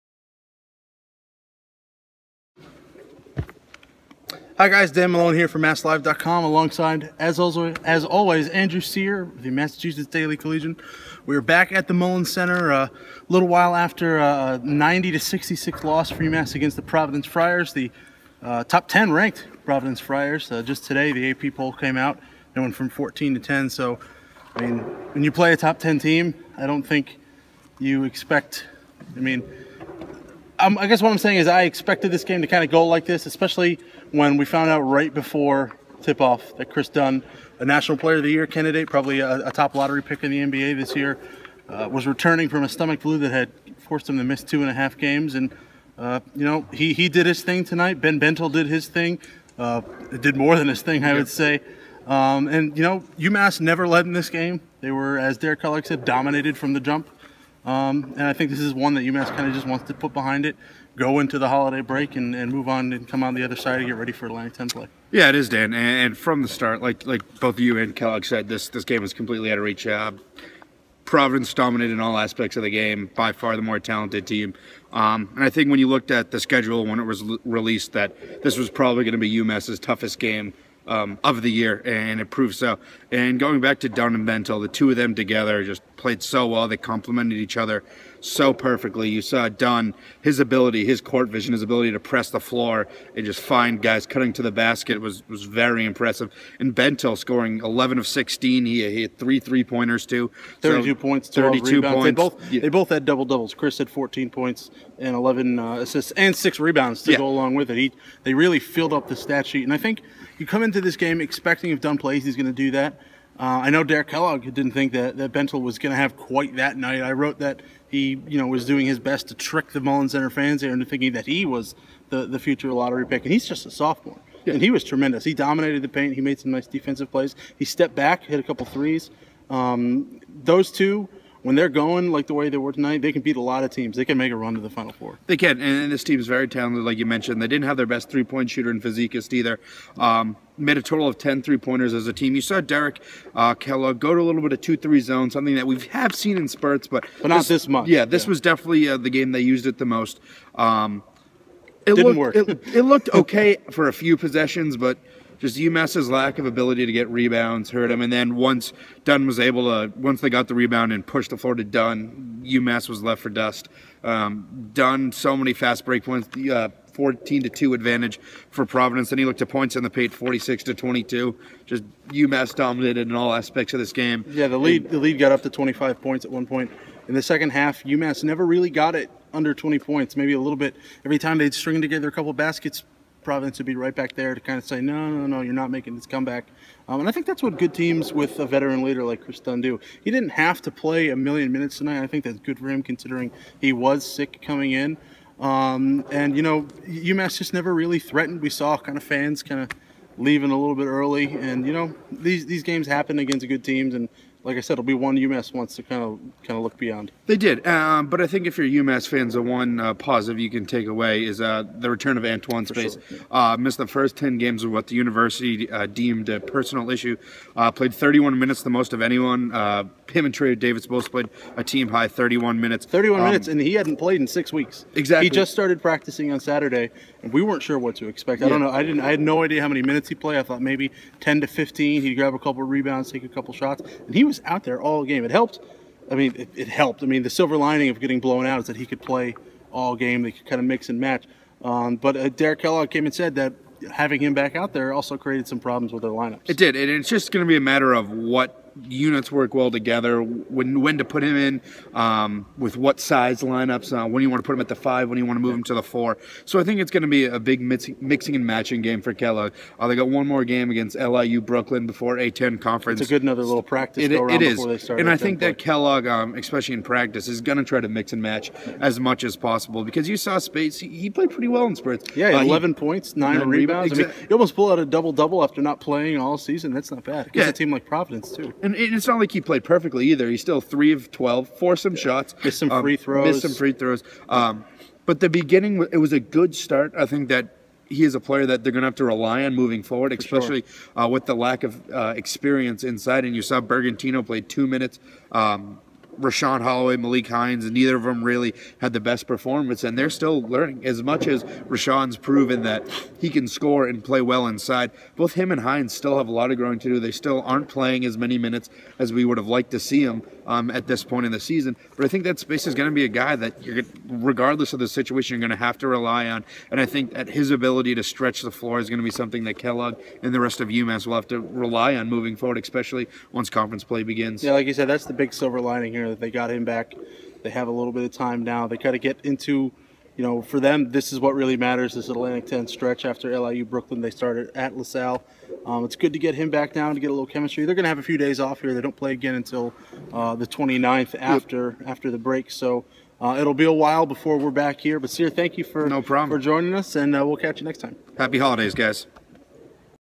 Postgame Analysis: Dan Malone & Andrew Cyr Break Down UMass' Loss To Providence
UMass writers share their thoughts on the Minutemen's 90-66 loss on Monday.